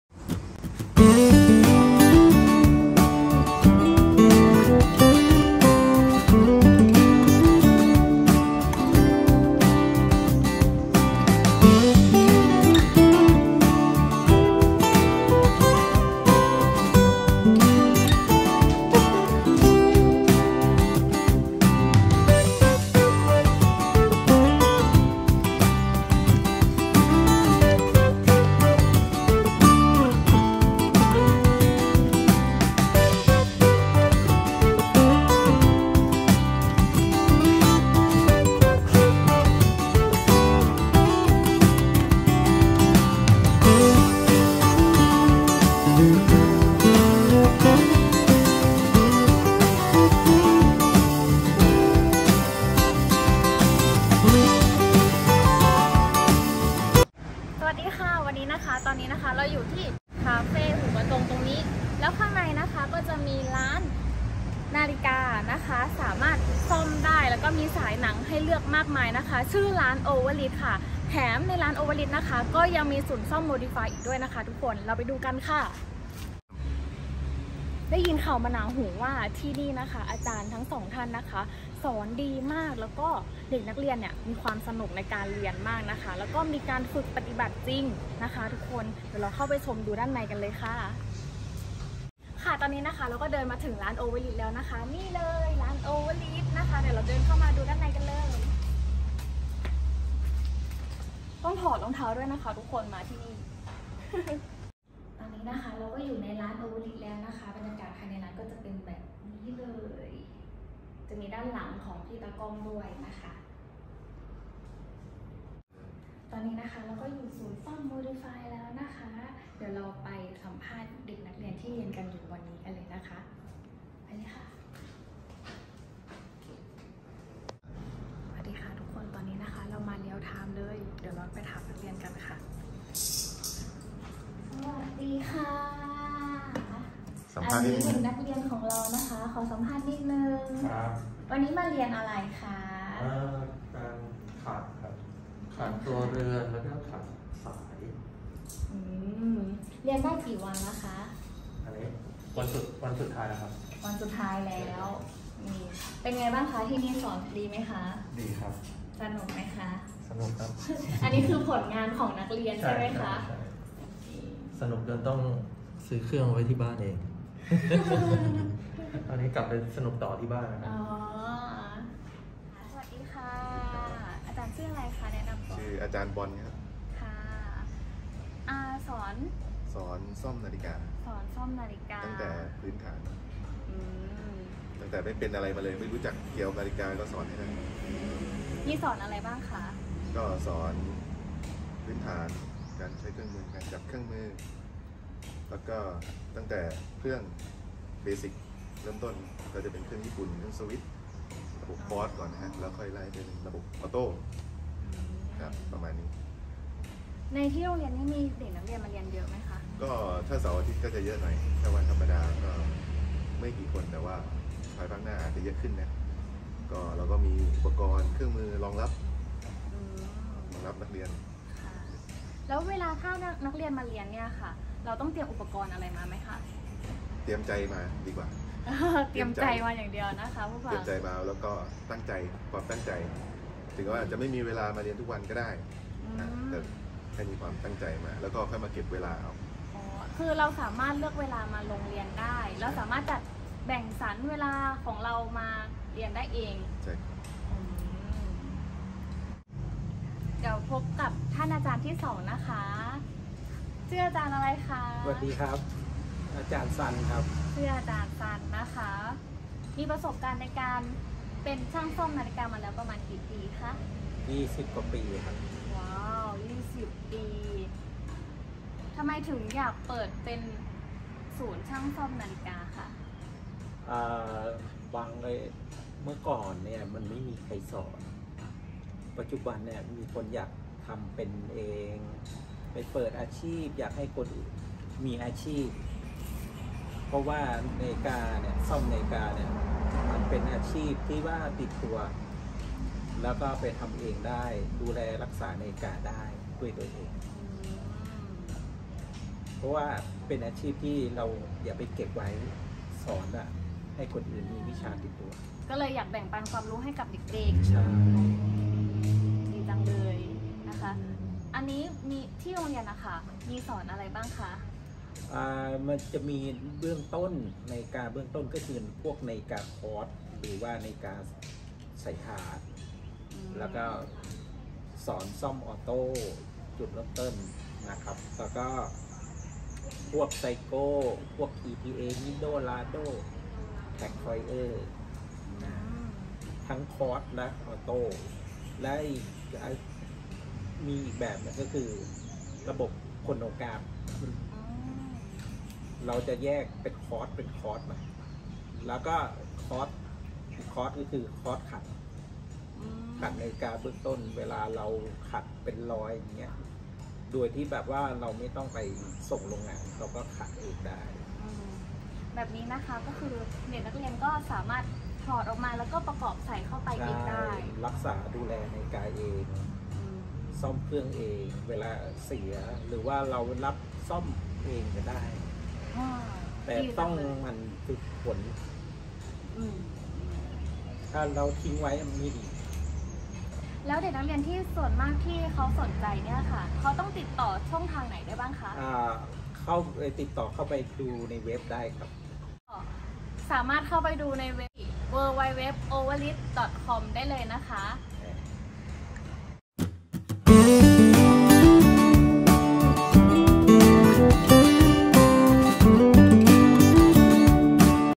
ศูนย์ฝึกอบรมช่างซ่อมนาฬิกา
สอนปฎิบัติ วิชาซ่อมนาฬิกา สำหรับคนที่ไม่มีพื้นฐาน สนใจดูรายละเอียดเพิ่มเติมได้ที่